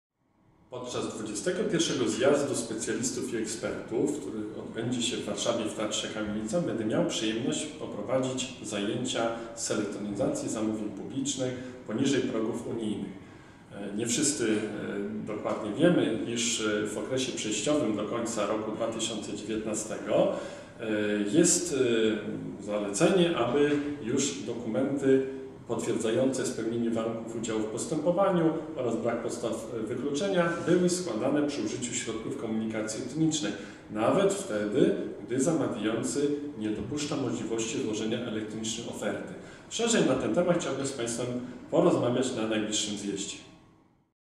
XXI Zjazd Specjalistów i Ekspertów ds. Zamówień Publicznych - Grzegorz Czaban
Zapraszamy do obejrzenia zapowiedzi panelu eksperckiego trenera ApexNet - Grzegorza Czabana, który podczas swojego wystąpienia będzie mówił o elektronizacji zamówień publicznych poniżej progów UE w okresie przejściowym (do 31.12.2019 r.).

Chcesz wziąć udział w Zjeździe? Zapraszamy do zapisów!